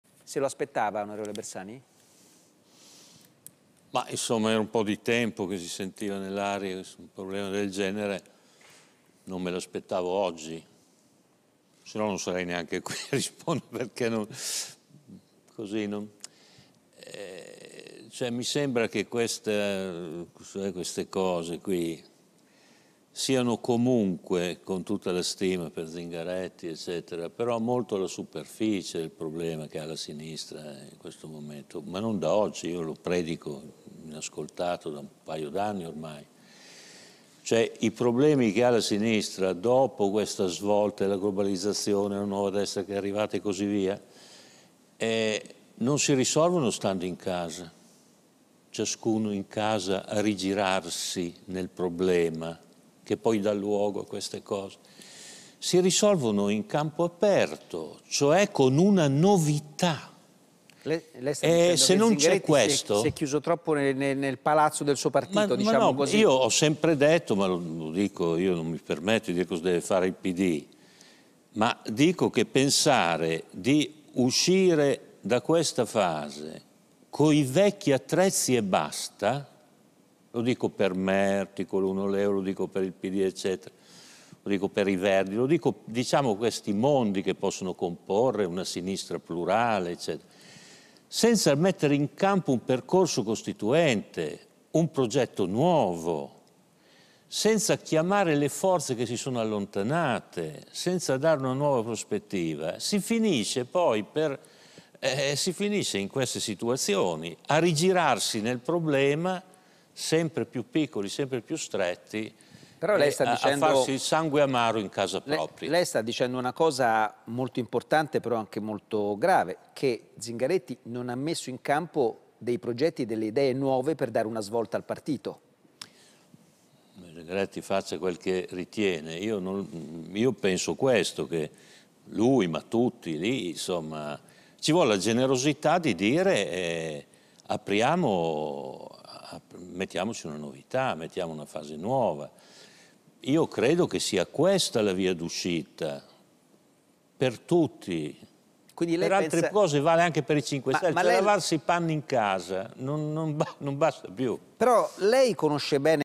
Le dimissioni di Zingaretti, Bersani: “Alla sinistra serve una fase nuova, per tutti”
Pier Luigi Bersani: “Le dimissioni di Zingaretti? Erano nell’aria da un po’ ma non mi aspettavo succedesse oggi. Ma siamo ancora molto sulla superficie dei problemi della sinistra: questi non si risolvono stando in casa ma in campo aperto, con una novità. Ci vuole la generosità di dire: mettiamoci una novità, mettiamo una fase nuova. Credo che sia questa la via d'uscita. Per tutti”.